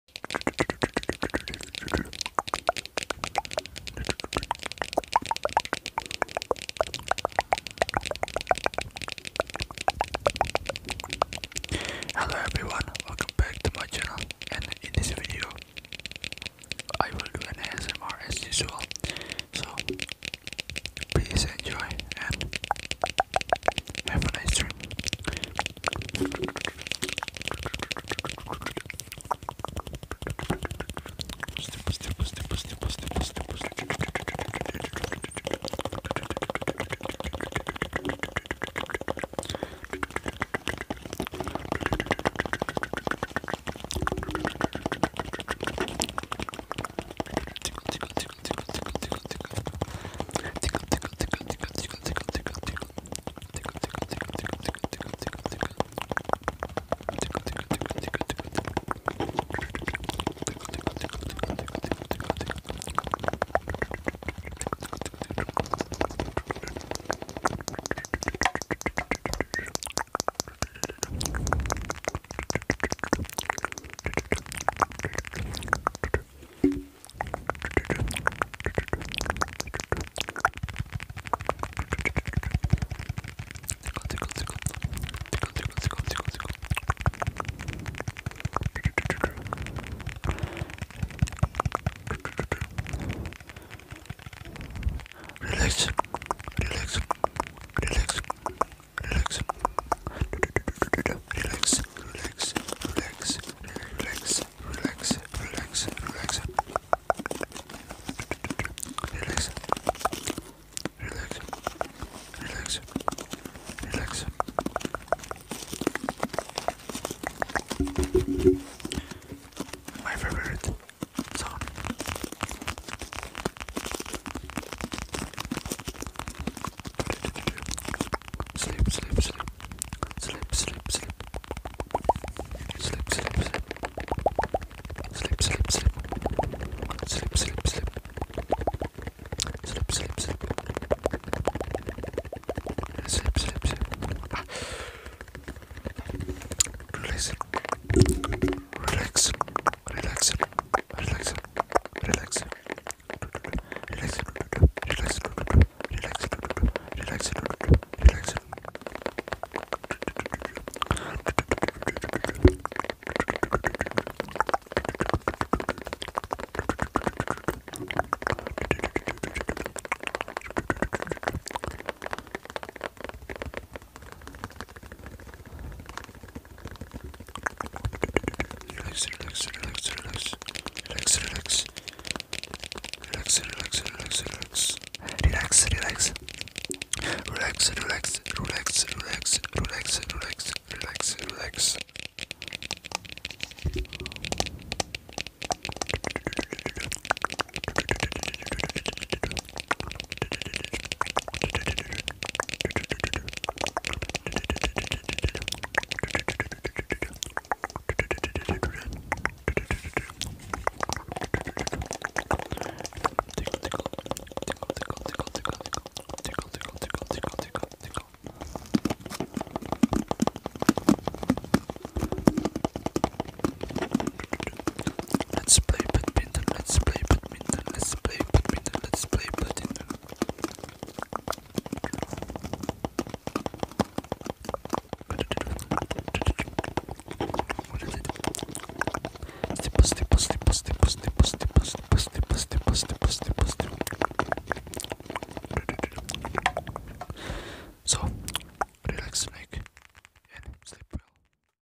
ASMR / Double Power Tapping and Mouth Sound 4 Minute
ASMR Double Power Tapping and Mouth Sound.

Hope you guys enjoy my Content.

I'm sorry if there is a disturbing or something that make you guys uncomfortable by this ASMR. And have a nice dream!

If you have sleep/anxiety/psychological problem please consult physician.